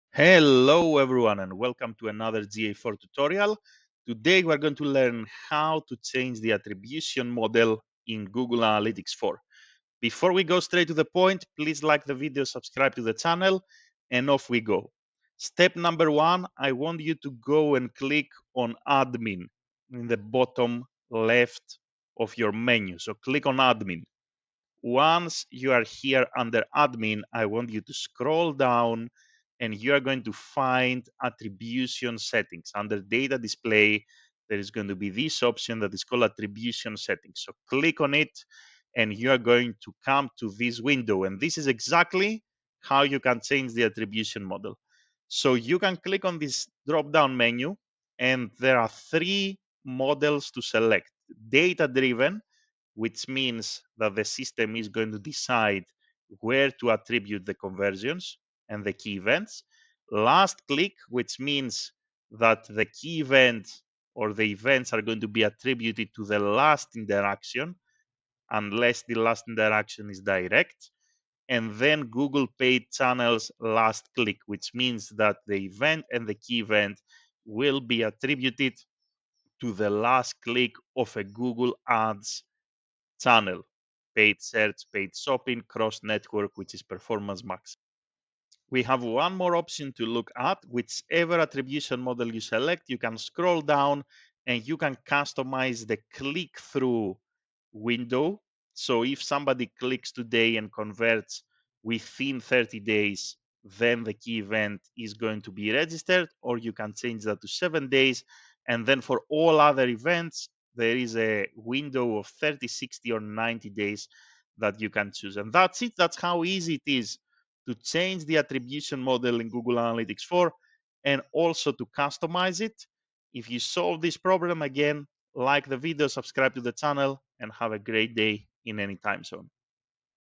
How to Change Attribution Model in Google Analytics
1) Navigate to Admin (bottom-left-hand-side)
2) Scroll down and under Data Display select Attribution settings

3) Choose one of the following Attribution models:
 - Data Driven (Paid and organic channels)
- Last Click (Paid and organic channels)
- Last Click (Google paid channels)

4) Scroll down to customise the Click-through window for Key Events and Events

That's it!